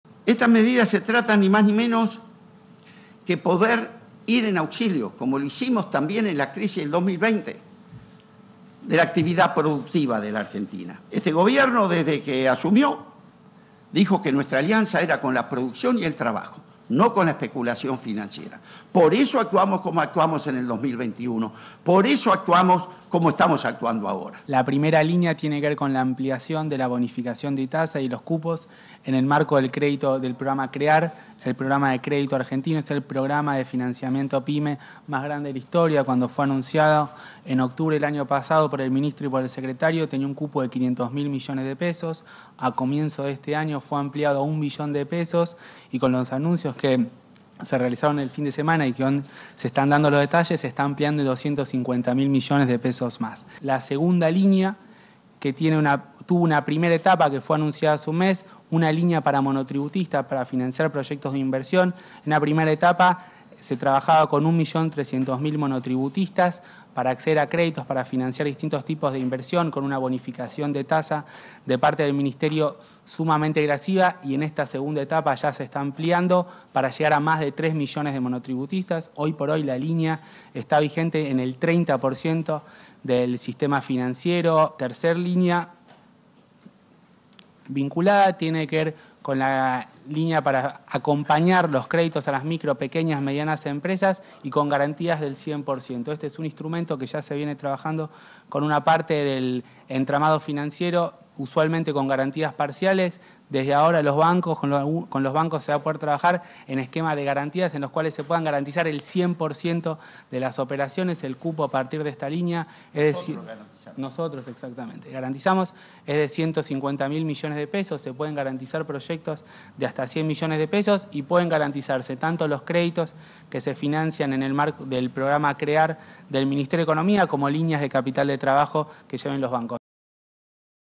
INDUSTRIA ANUNCIÓ $ 250.000 MILLONES EXTRA EN CRÉDITOS
JOSÉ IGNACIO DE MENDIGUREN - El secretario de Industria y Desarrollo Productivo detalló las medidas para sectores industriales y pymes anunciadas por Sergio Massa. Confirmó $ 250,000 millones extras en créditos para pymes.